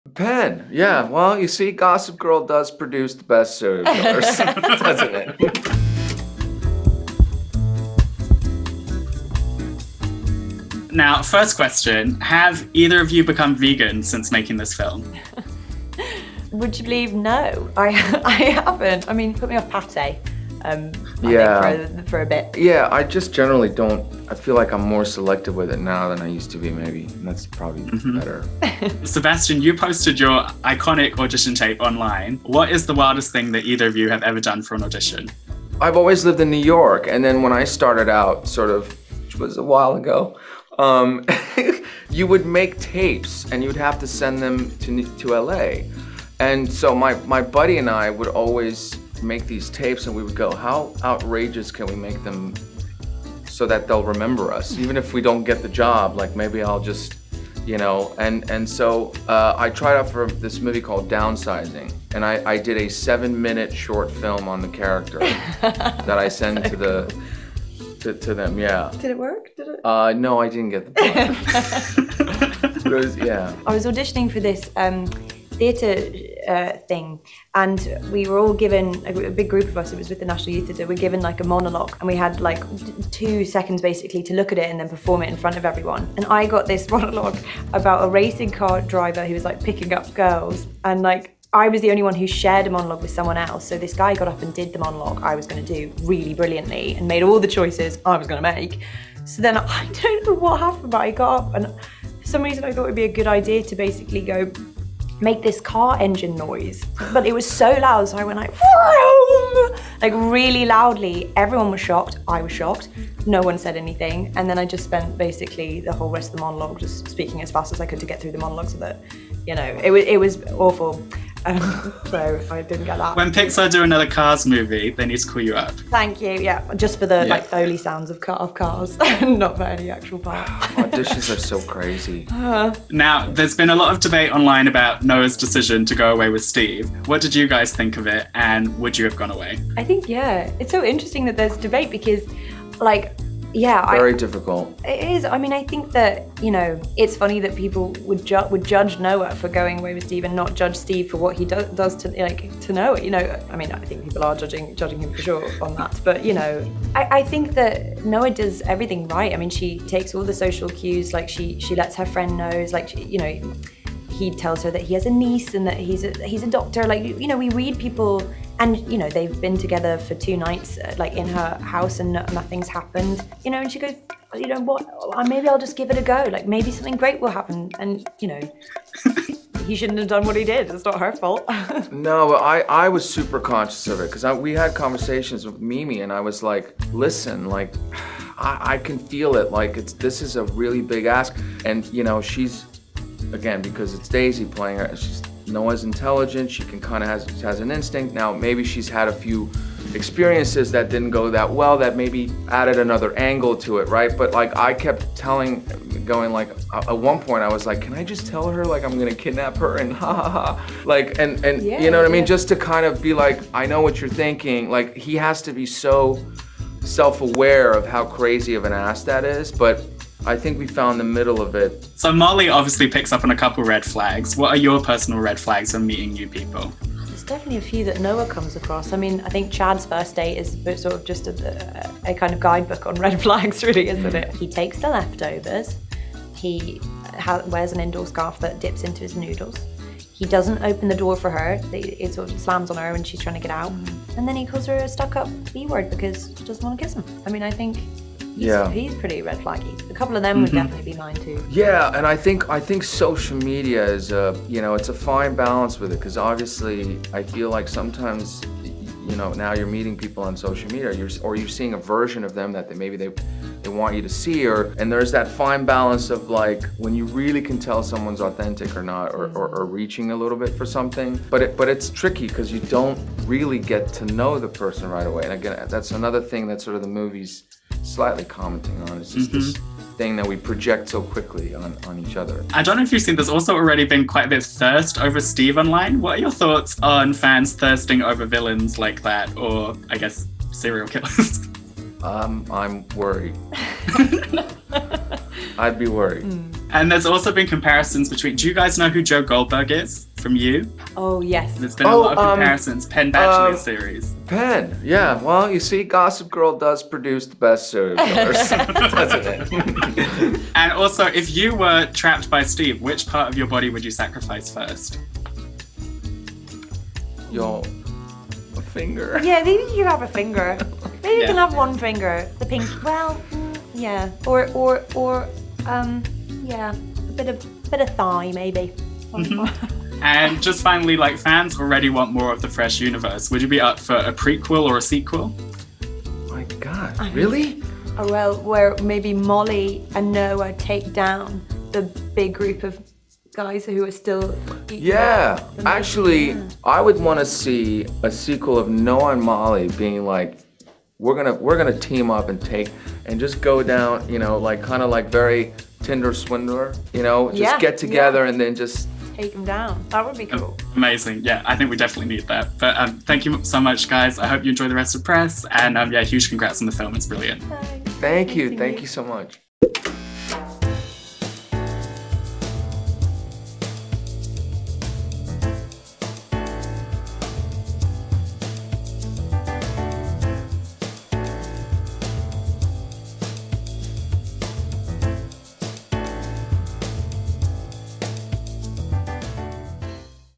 Sebastian Stan Talks Gossip Girl Serial Killers & FRESH Sequel With Daisy Edgar-Jones
In honour of the UK release of 'Fresh' 🥩 we spoke to Sebastian Stan and Daisy Edgar-Jones about crushing on serial killers, comparisons between Steve and Joe Goldberg from You, and what they'd like to see in a Fresh sequel 👀

V I D E O  P L A Y L I S T S: